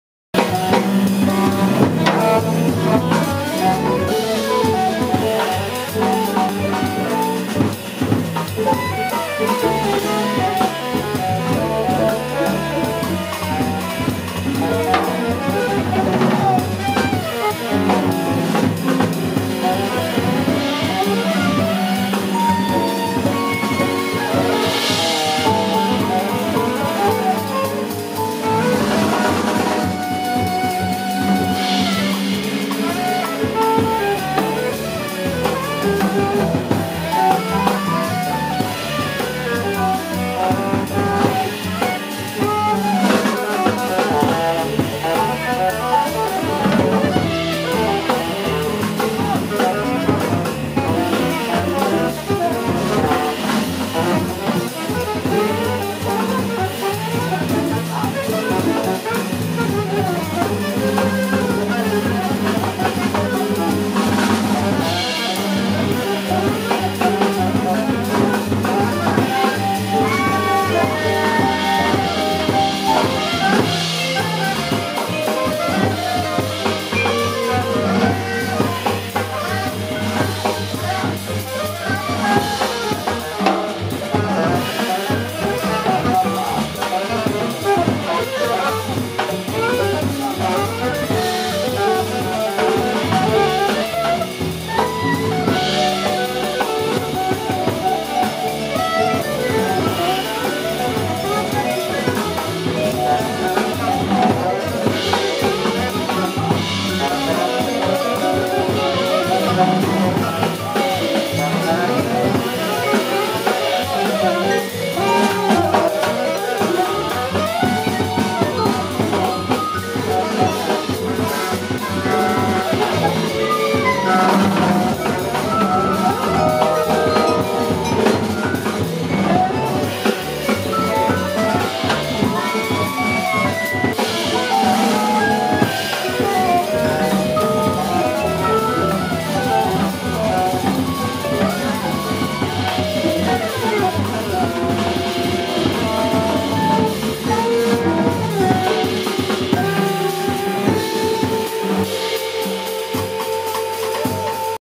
Joel Ross Good Vibes at Smalls Jazz Club, New York 2017
Joel Ross (vibraphone) with Immanuel Wilkins (alto sax) - Fabian Almazan (piano) - Harish Raghavan (bass) & Marcus Gilmore (drums) live